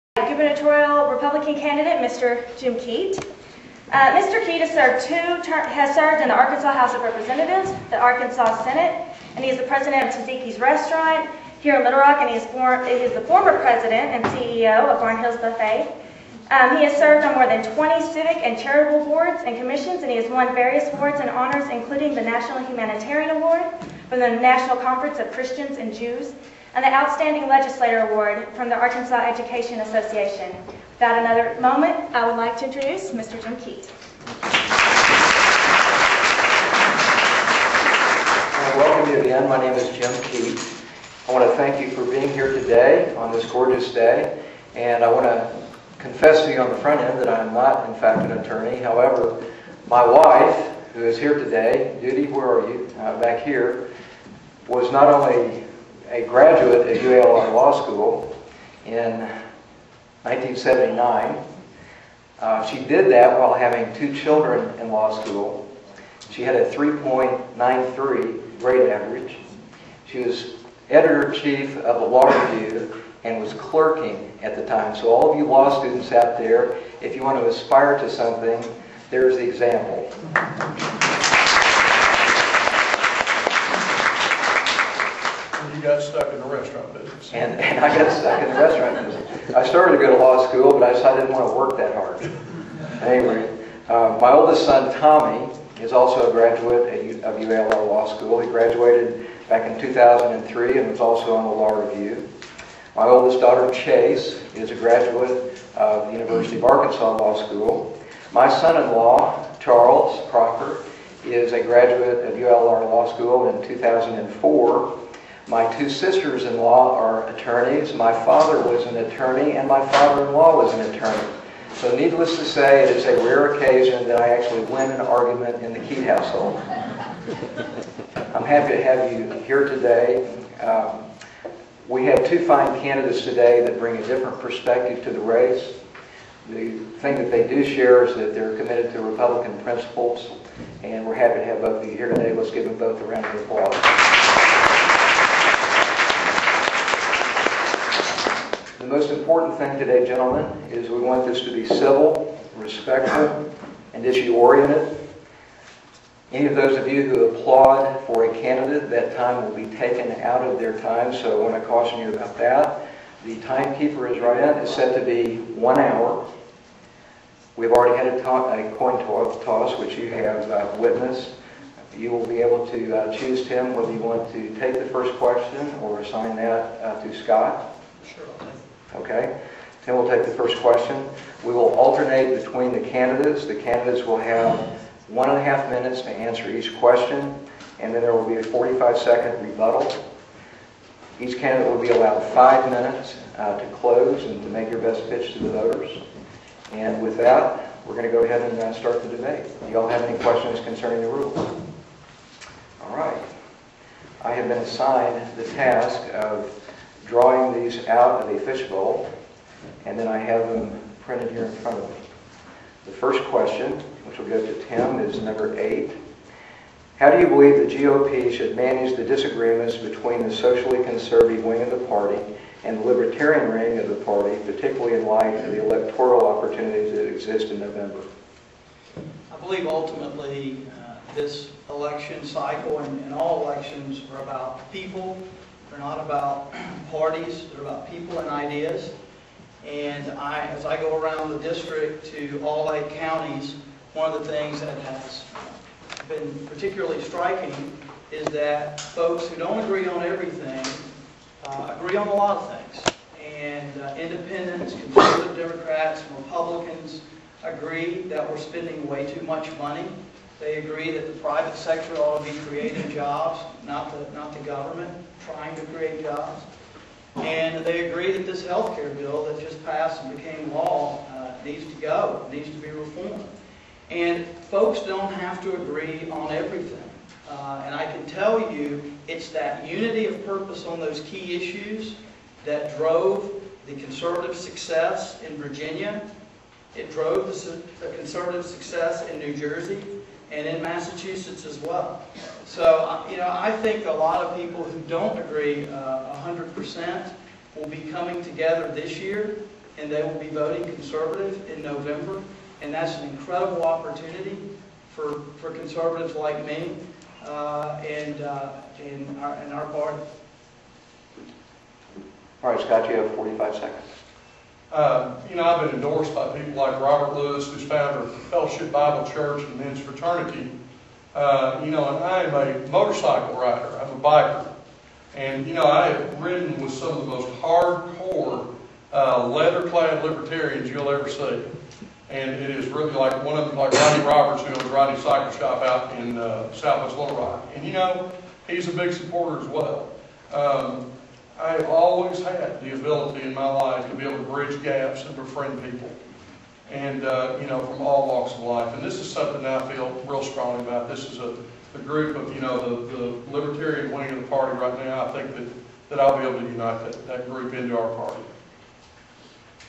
Arkansas 2nd Congressional Debate 01.wmv
Candidates Scott Wallace and Tim Griffen debated at the University of Arkansas School of Law for the position of Republican Nominee to the 2nd United States Congressional District.